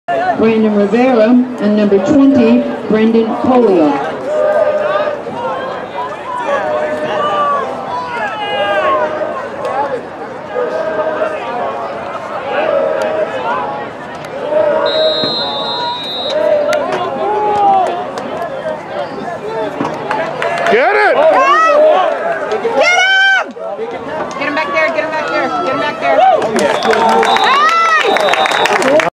Rutherford HS v Lyndhurst Game 1 10 2 2020 071 Ryan 4th Kickoff
Ryan Ward Rutherford NJ RHS Football Kicker & Receiver